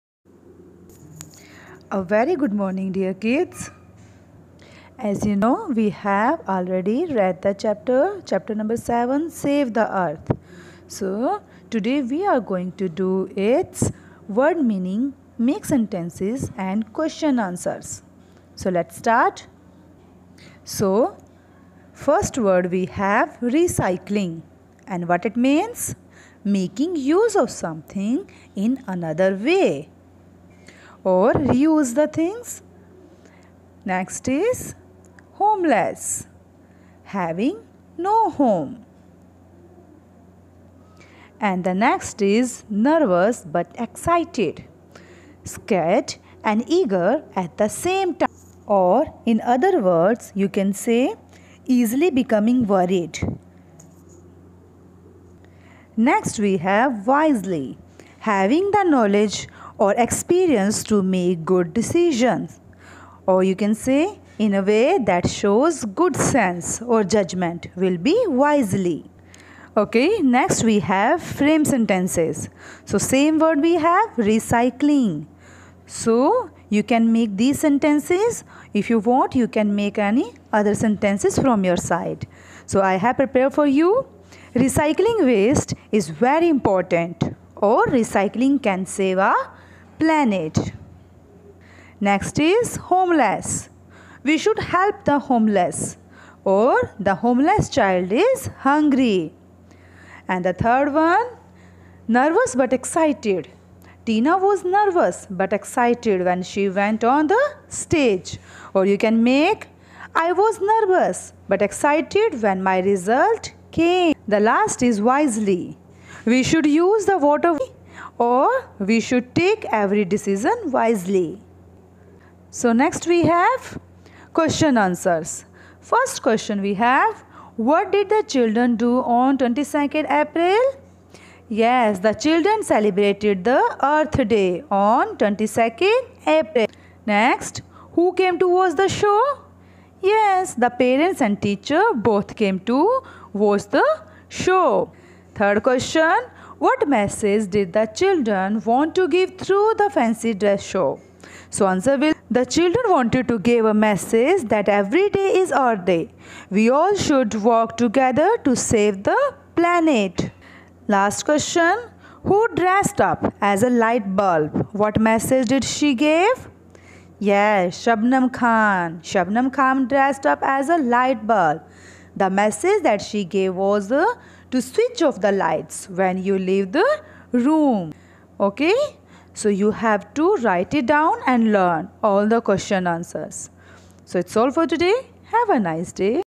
Question answer of Save the Earth
Explanation of Word meaning, make sentences and question answer of Lesson-7